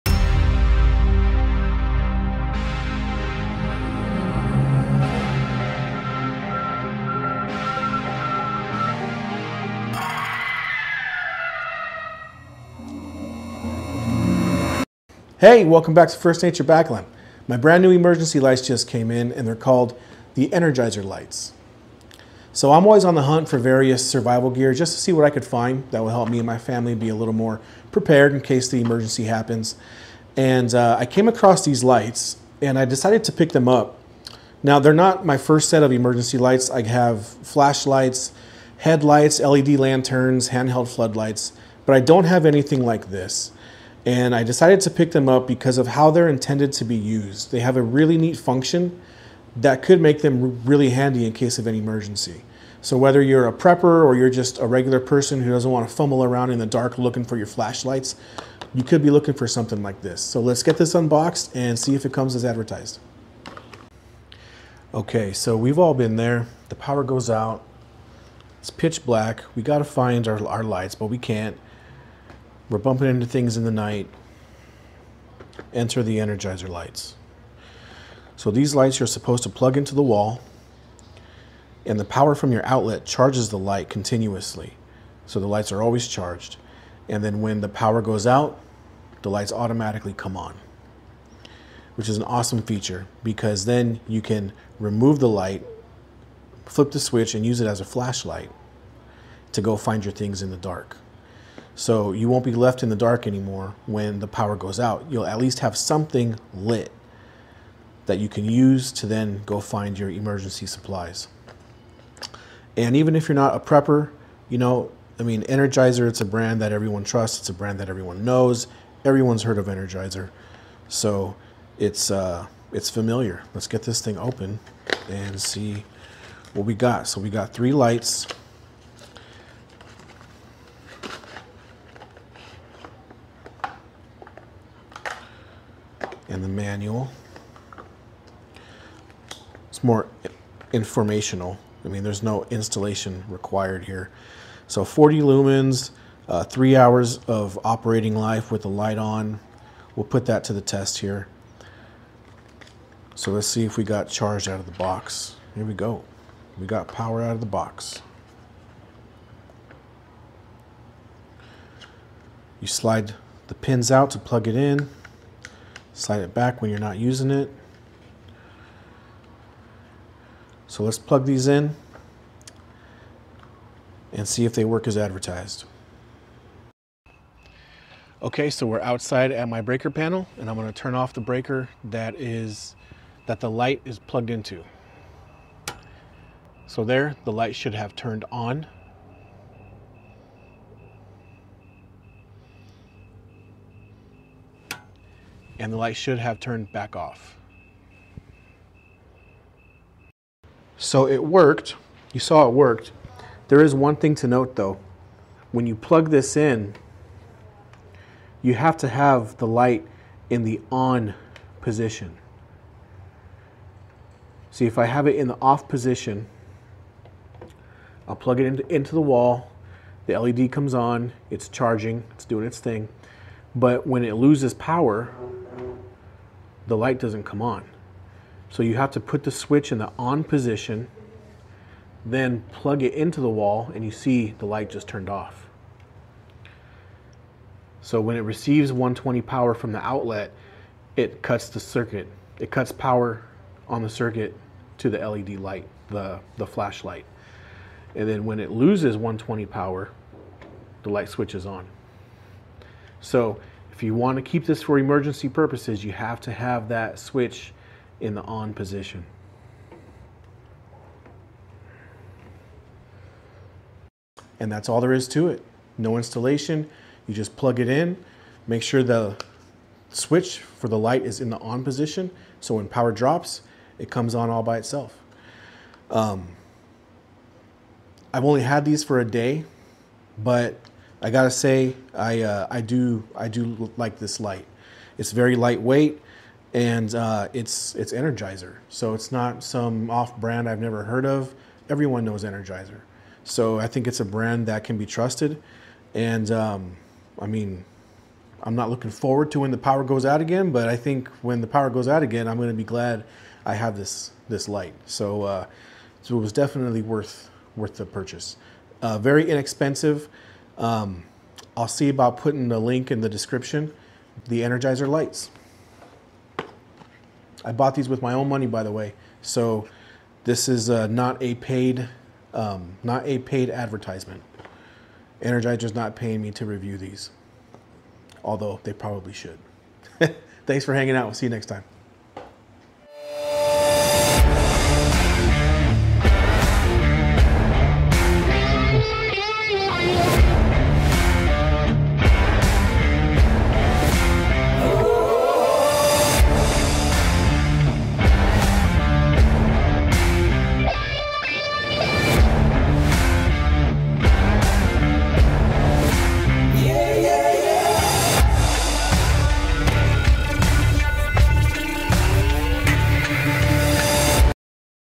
Automatic Lights When The Power Goes Out - No More Fumbling In The Dark
When the power goes out it’s hard to find your supplies in the dark. These flashlights plug into the wall and turn on automatically when the power is lost. Then you can unplug them from the wall and use them to find your emergency supplies. No more fumbling around in the dark! The #1 item at the top of everyone’s emergency supply list is a flashlight. Check out these flashlights that illuminate when the power goes out.

❤️Love our content? It costs you nothing, but it helps us a ton! Just use this link whenever you want to shop on Amazon. Thanks for being outstanding!

(Links to items in this video)
Energizer Lights

Intro 00:00
Unboxing 01:04
Test 02:57
How to Use: 03:20
Recommendation 04:23
Purchase 05:08
Outro 05:33